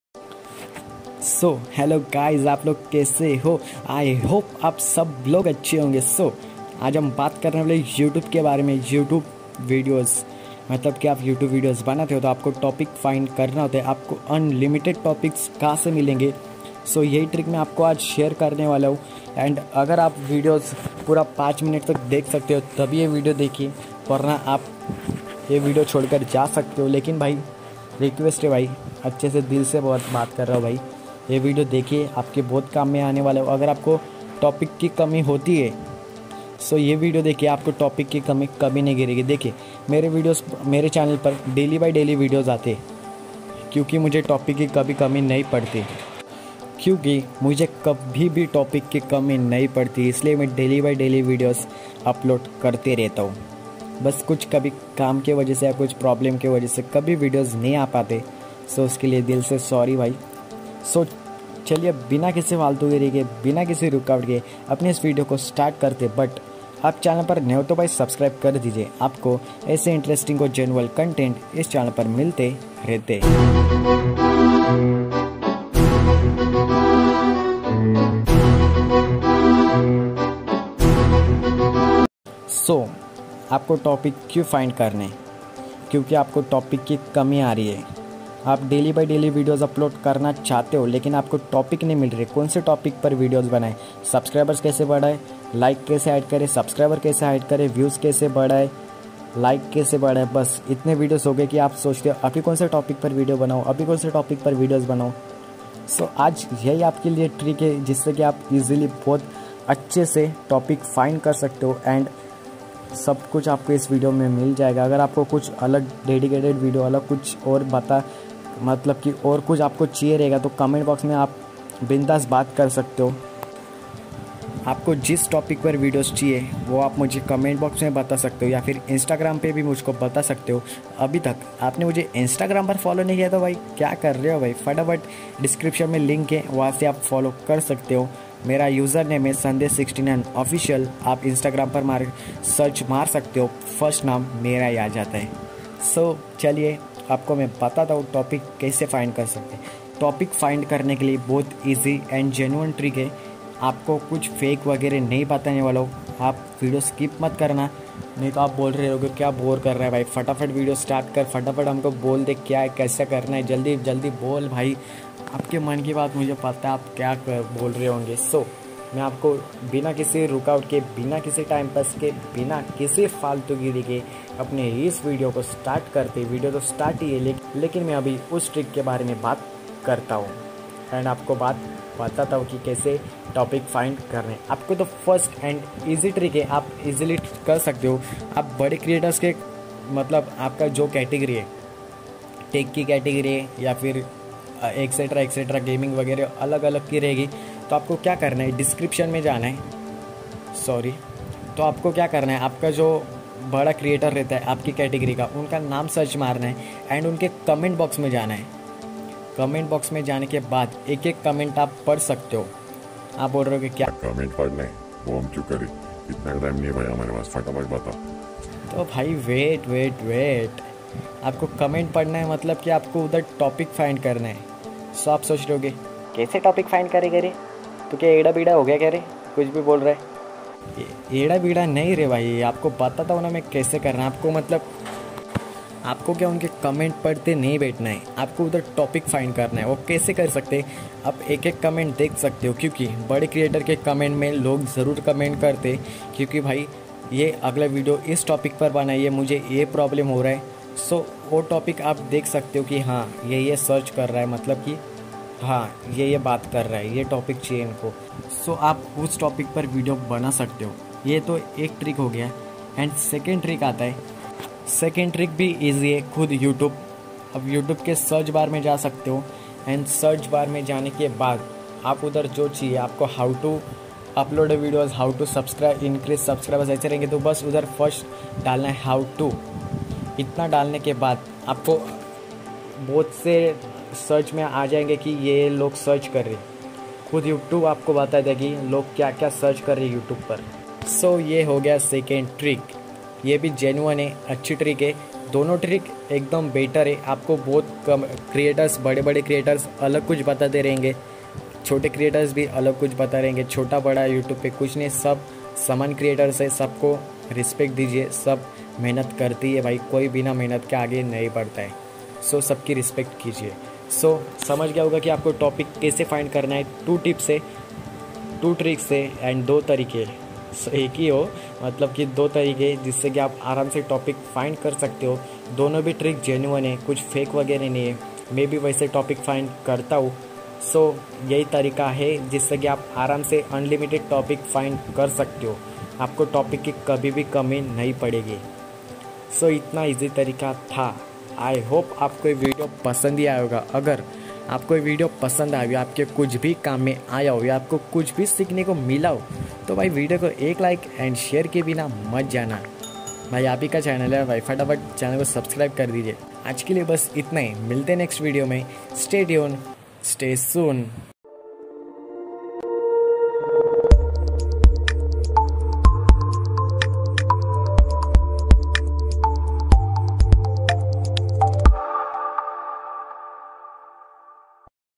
How To Find Unlimited Topics From YouTube Videos | Genuine Trick | video topics kaise find kare
Hello guy's
My Name Sandesh Sanjay Shejwalkar
You're Watching Our YouTube Channel YTSearch

Today We Learn : How To Find Unlimited Topics From YouTube Videos | Genuine Trick | video topics kaise find kare